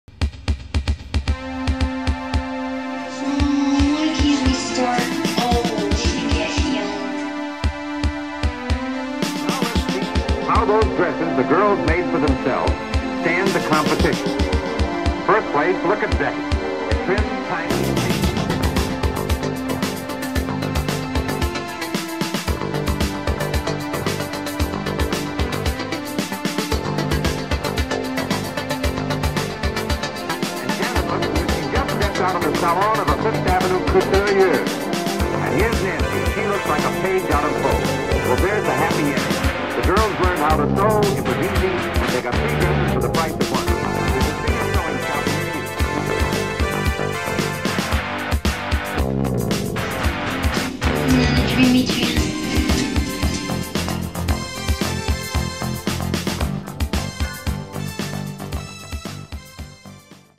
MAKING MY OWN BUTTERFLY THEMED HOMECOMING DRESS - Pretty In Pink Inspired Video
(this is for my portfolio)